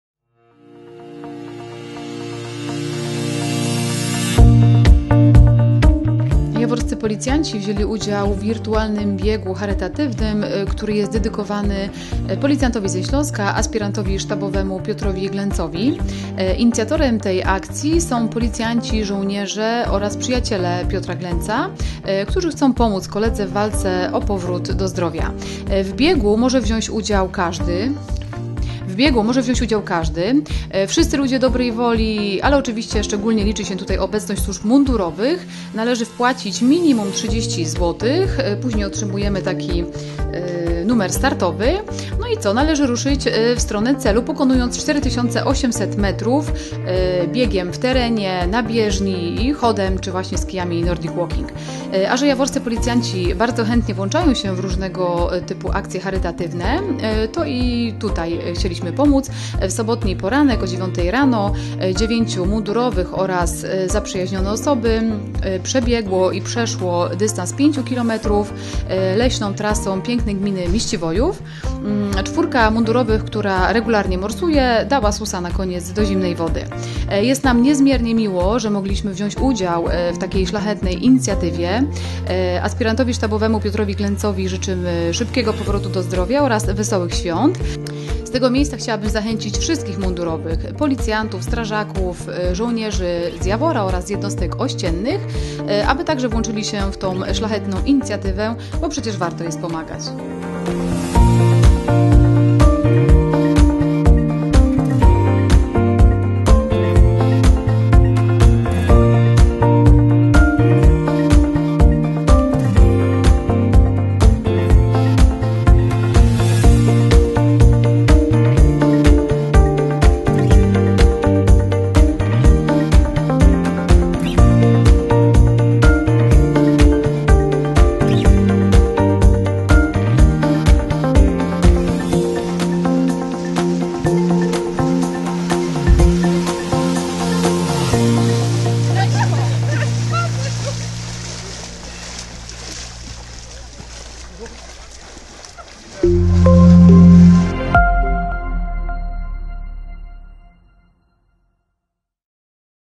JAWOR. Policyjna akcja charytatywna
Na portowo! - policyjna akcja charytatywna dla chorego funkcjonariusza ze Śląska